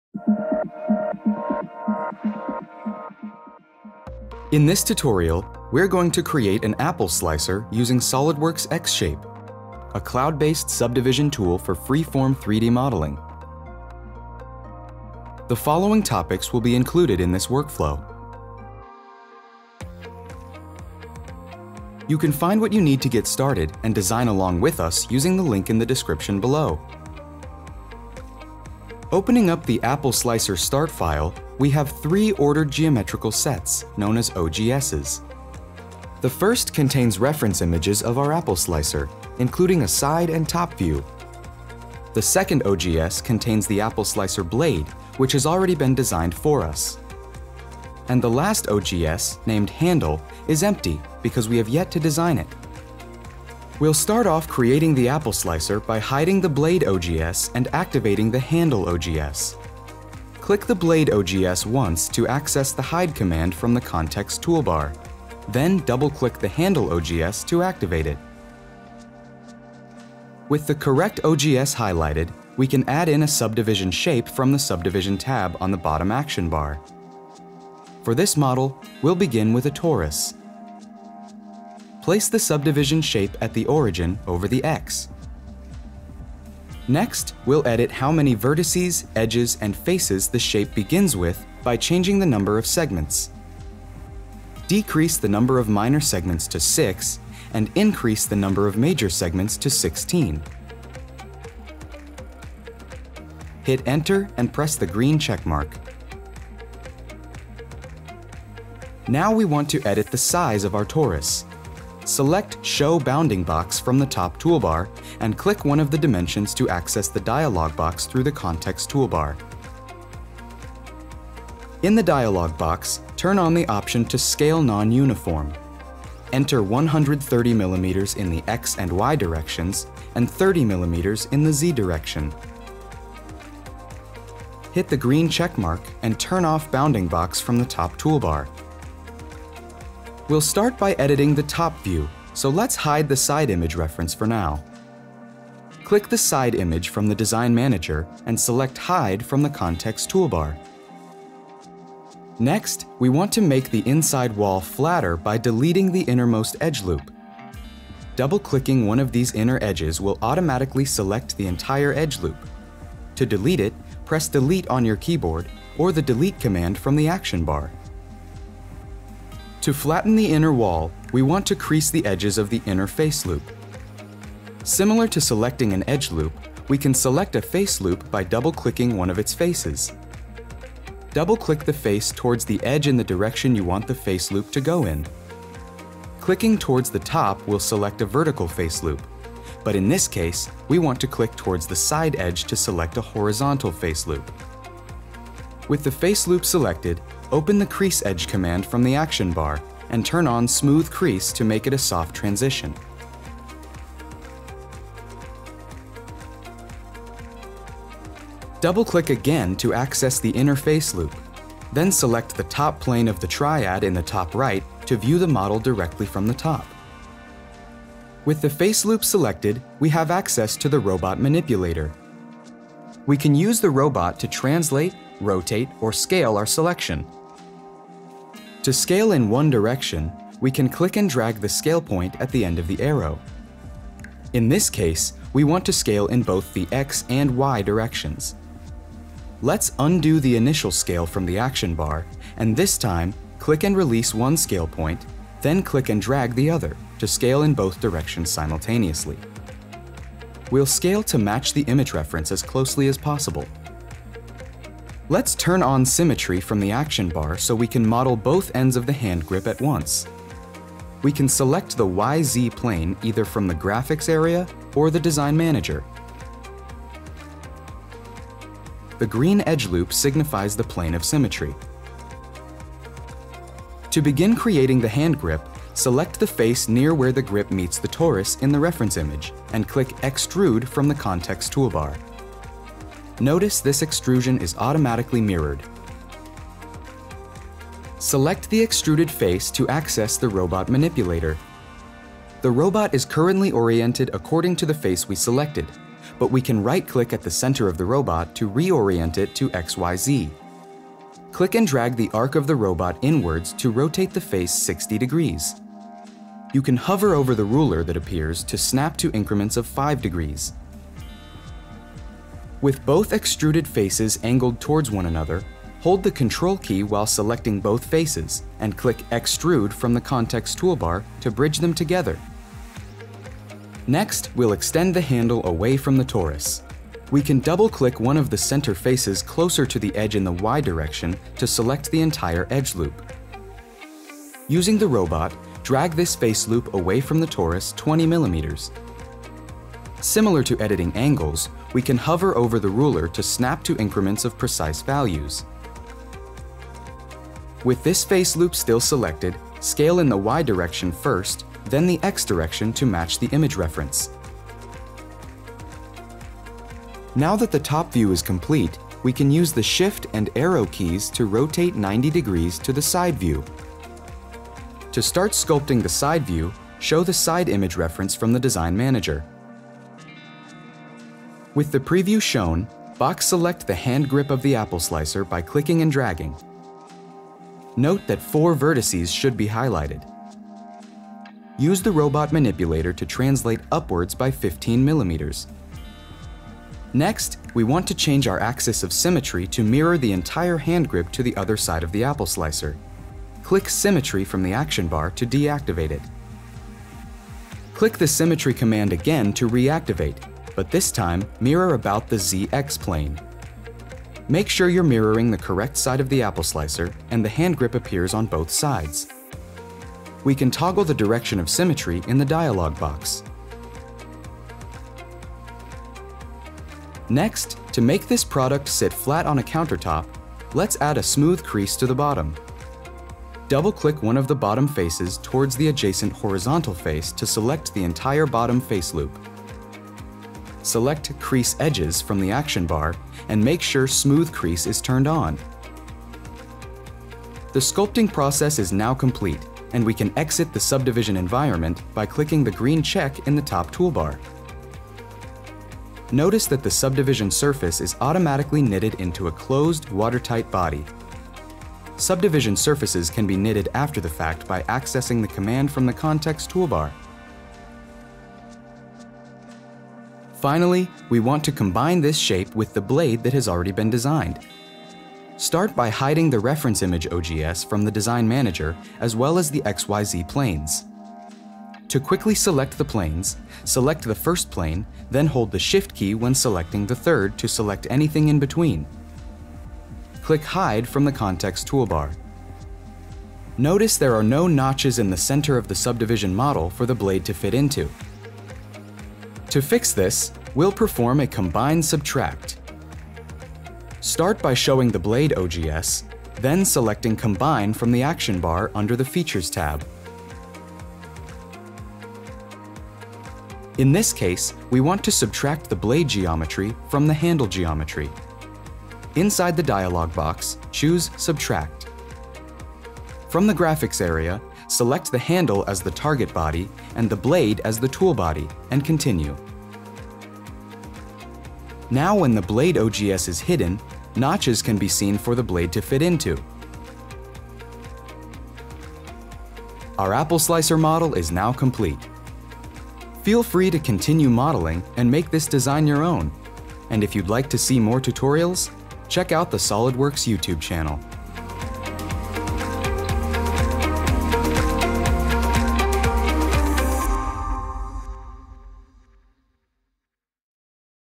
How to Design an Apple Slicer with Subdivision Modeling - Made in SOLIDWORKS xShape
Learn how to design an apple slicer with SOLIDWORKS xShape, found in 3D Sculptor on the 3DEXPERIENCE platform.

*Download the files needed to follow along with this tutorial using this link:*

xShape is a next-generation subdivision modeling tool that runs entirely in your browser. It takes a freeform approach to creating complex shapes and surfaces. You can design 100% in the cloud or seamlessly integrate your xShape models with your SOLIDWORKS designs. Follow along with this tutorial to become a modeling master.

Looking to buy xShape professionally?

Looking to buy xShape for personal use?

00:00- Intro
00:30 - Start File (3DXML)
01:00 - Hide/Activate OGS
01:10 - Insert Sub-D Torus Primitive
01:26 - Adjust Segments of Torus
01:48 - Scale by Bounding Box
02:20 - Hide Image Reference
02:32 - Delete Edge Loop
02:49 - Add Smooth Crease Edges
03:28 - Scale in Two Directions
04:14 - Symmetry
04:33 - Extrude Face
04:47 - Re-orient Robot to XYZ
05:00 - Rotate with Robot
05:12 - Bridge Faces
05:23 - Translate with Robot
05:47 - Scale in a Single Direction
05:56 - SHIFT & Arrow Keys to Rotate View
06:04 - Show Image Reference
06:10 - Box Selection, Robot Translation
06:26 - Change Axis of Symmetry
06:56 - Add Smooth Crease Edges
07:16 - Exit Subdivision Environment
07:25 - Knit Surface
07:45 - Hide OGS & XYZ Planes
08:03 - Combine/Subtract Feature
08:49 - Final Shape Complete